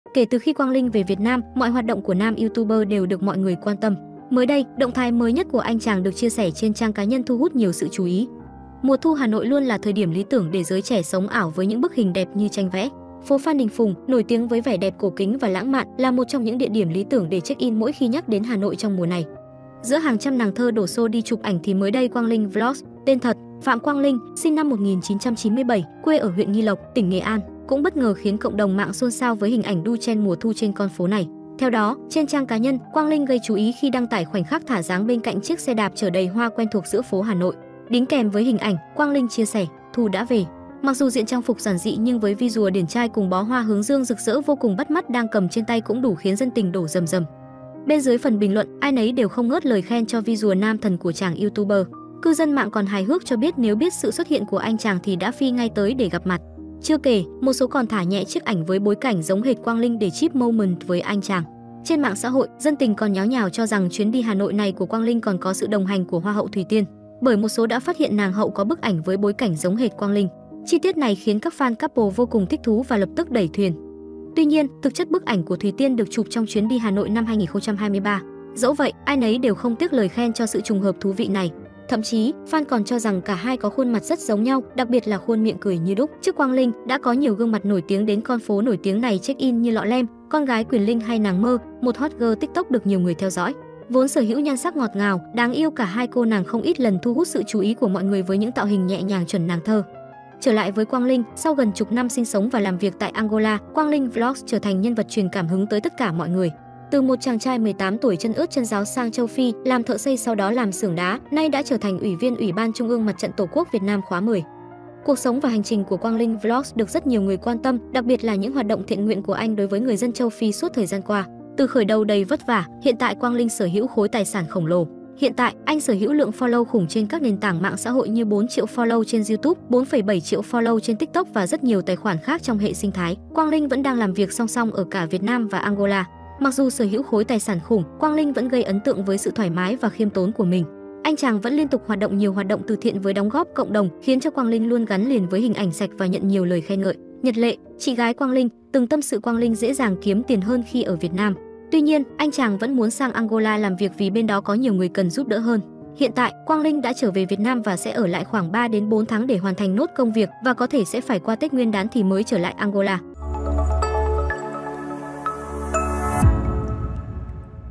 Quang Linh ra Hà Nội “đu trend”, Thùy Tiên đi cùng?
Kể từ khi Quang Linh về Việt Nam, mọi hoạt động của nam Youtuber đều được mọi người quan tâm. Mới đây, động thái mới nhất của anh chàng được chia sẻ trên trang cá nhân thu hút nhiều sự chú ý.

Mùa thu Hà Nội luôn là thời điểm lý tưởng để giới trẻ "sống ảo" với những bức hình đẹp như tranh vẽ. Phố Phan Đình Phùng, nổi tiếng với vẻ đẹp cổ kính và lãng mạn, là một trong những địa điểm lý tưởng để check-in mỗi khi nhắc đến Hà Nội trong mùa này.

Giữa hàng trăm nàng thơ đổ xô đi chụp ảnh thì mới đây Quang Linh Vlogs (tên thật: Phạm Quang Linh, SN 1997, quê ở huyện Nghi Lộc, tỉnh Nghệ An) cũng bất ngờ khiến cộng đồng mạng xôn xao với hình ảnh “đu trend” mùa thu trên con phố này.